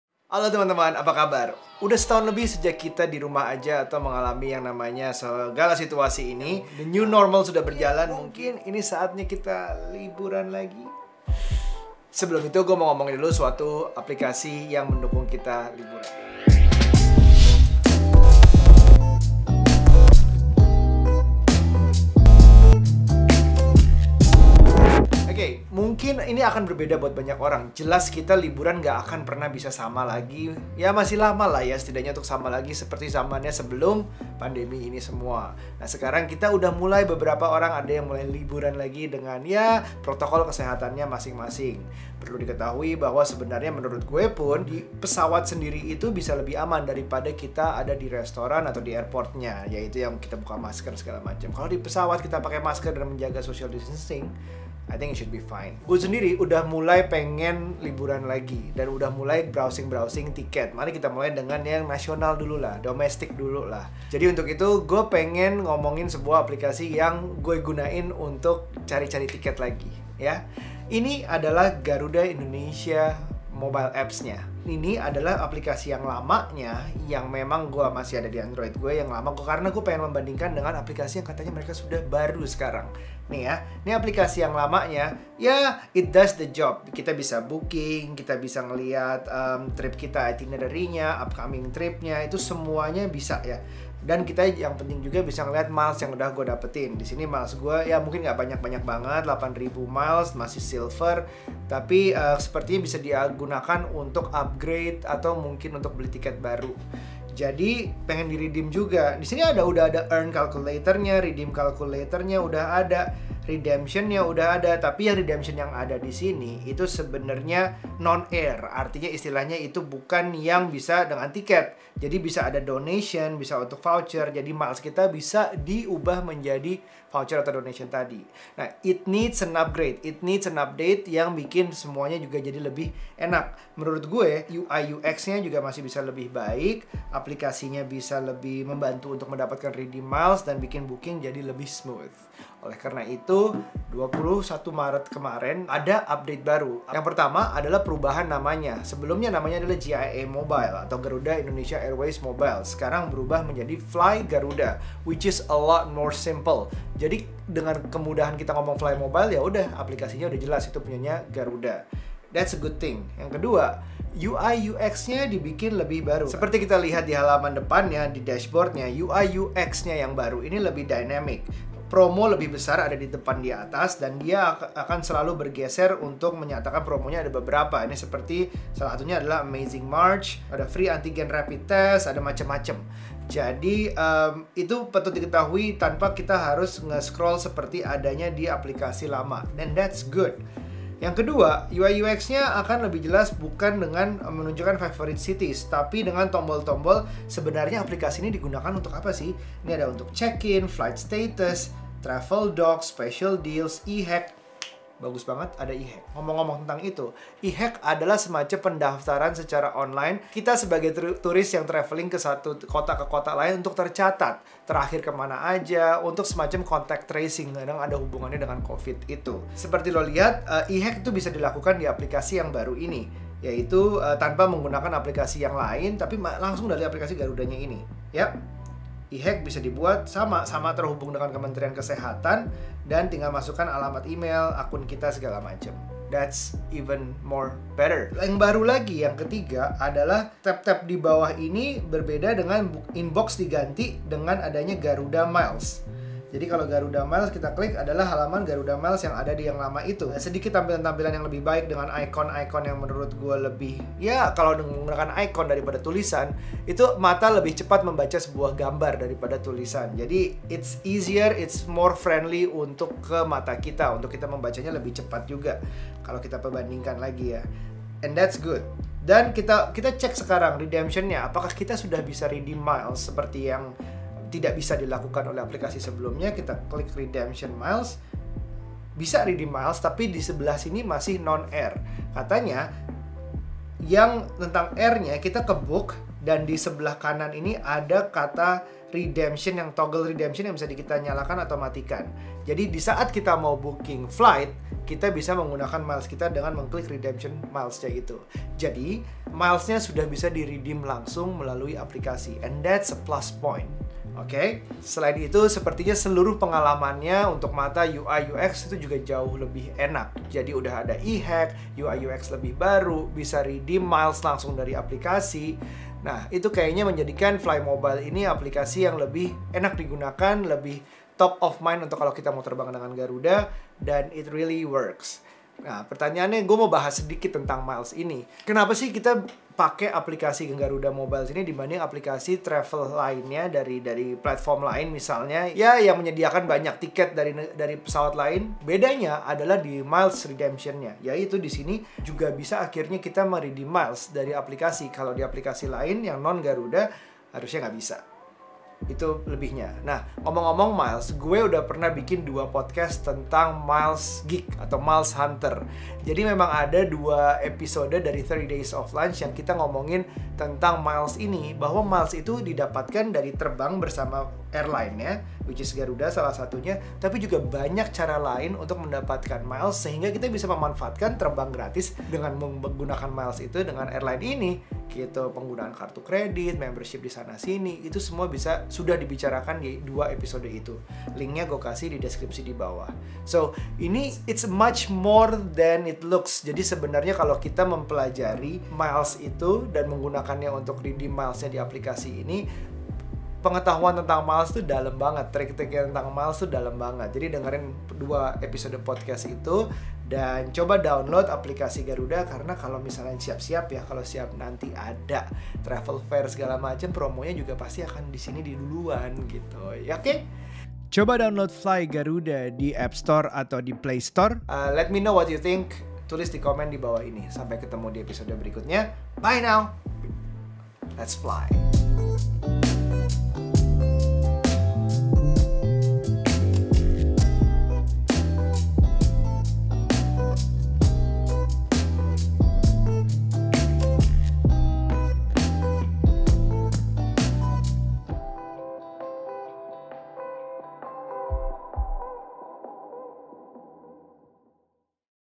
Fly Garuda: Aplikasi Baru Garuda Indonesia dan Kenapa Penting!
Fly Garuda adalah aplikasi terbaru dari Garuda Indonesia yang merupakan sebuah major update. Update besar ini memberikan perubahan pada UI/UX, dan juga sebuah fitur baru, Miles Redemption! Yay!

Follow gue di Instagram & tiktok @sheggario